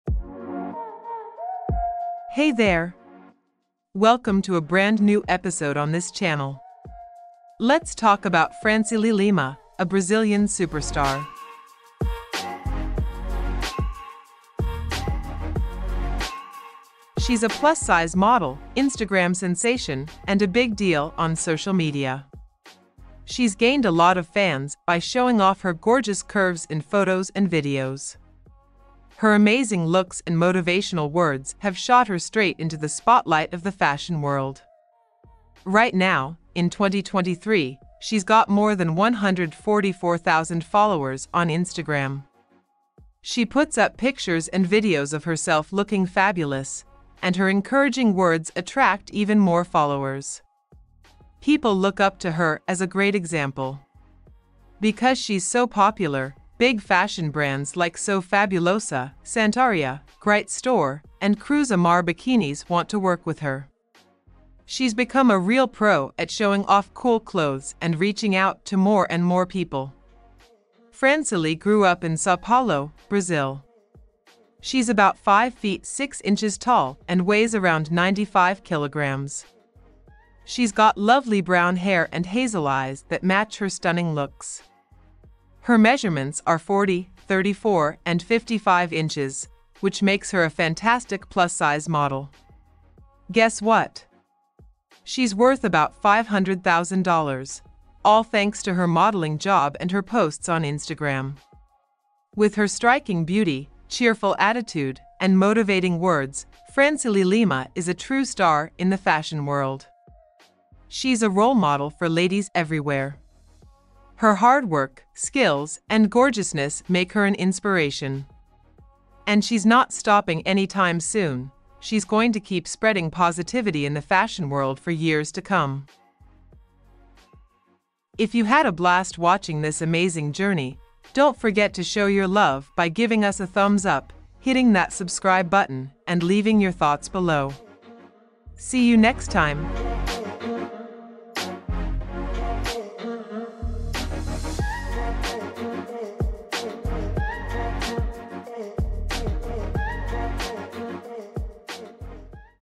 Francielly Lima: Brazilian Plus Size Model | Bio | Body Measurements | Height | Weight | Net Worth
Meet Francielly Lima, the Brazilian Plus Size Model breaking barriers in the fashion industry! In this video, we'll delve into her inspiring journey, learn about her body measurements, height, weight, and even discuss her impressive net worth. Don't miss this inspiring story of success and self-love